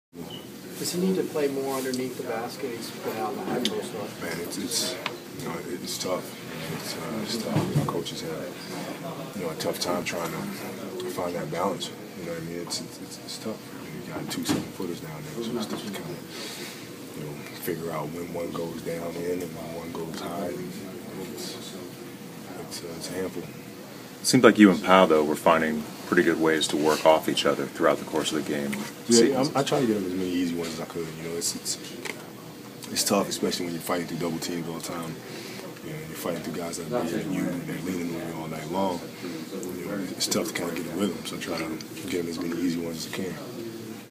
ESPNLA.com: Kobe on Pau's spots on the floor
Kobe Bryant talks about where Pau Gasol is getting his shots, and working together in Mike Brown's new offense.